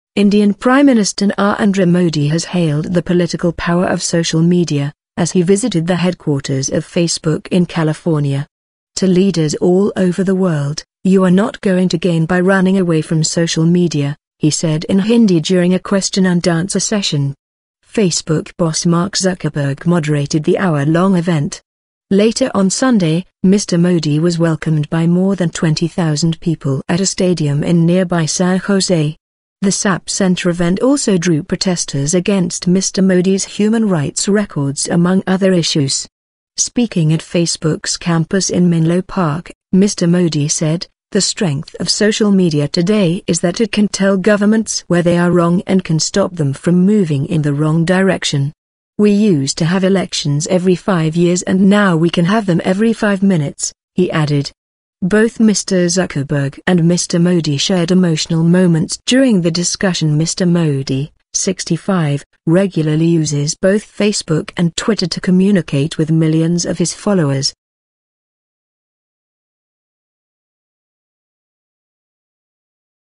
Facebook CEO Mark Zuckerberg meets Prime Minister Narendra Modi
Indian Prime Minister Narendra Modi has hailed the political power of social media, as he visited the headquarters of Facebook in California.

"To leaders all over the world: you are not going to gain by running away from social media," he said in Hindi during a question-and-answer session.

Facebook boss Mark Zuckerberg moderated the hour-long event.
Later on Sunday, Mr Modi was welcomed by more than 20,000 people at a stadium in nearby San Jose.

The SAP Center event also drew protesters against Mr Modi's human rights records among other issues.

Speaking at Facebook's campus in Menlo Park, Mr Modi said: "The strength of social media today is that it can tell governments where they are wrong and can stop them from moving in the wrong direction."

"We used to have elections every five years and now we can have them every five minutes," he added.

Both Mr Zuckerberg and Mr Modi shared emotional moments during the discussion

Mr Modi, 65, regularly uses both Facebook and Twitter to communicate with millions of his followers.